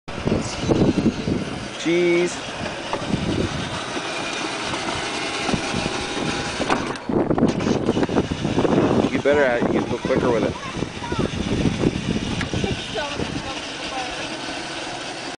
Fire Truck 2
Another Video of Dakota and Hunters fire truck